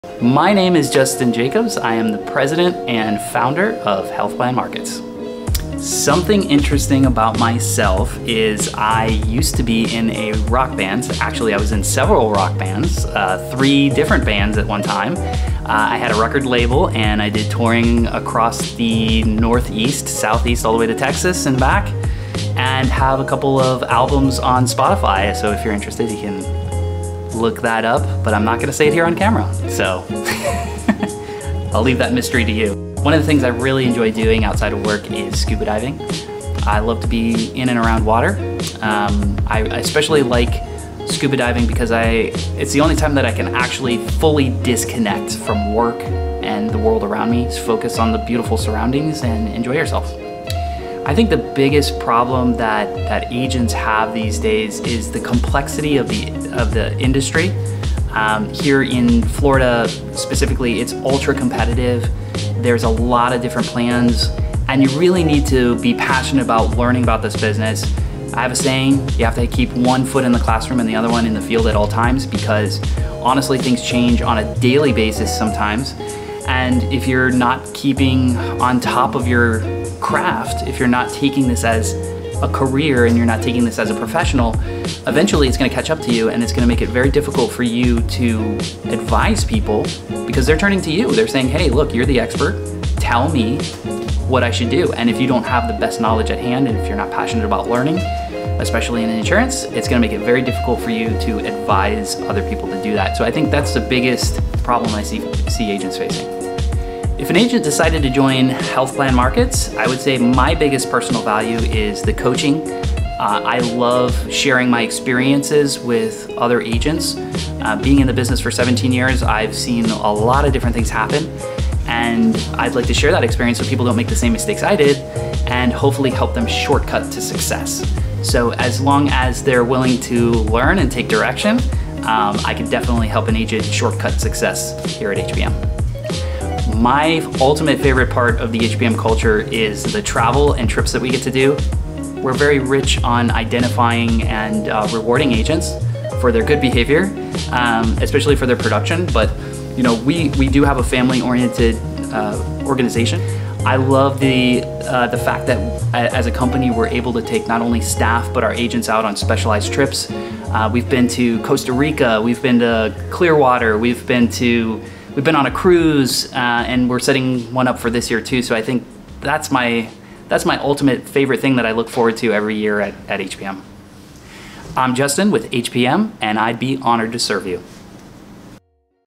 Meet the Team: Justin Jacobs, President
Do you know your FMO? We'd like to introduce our ourselves on an intimate-level so we can get to know each other a little better - Business is best when you're friends!

Introducing our founder & President, Justin Jacobs!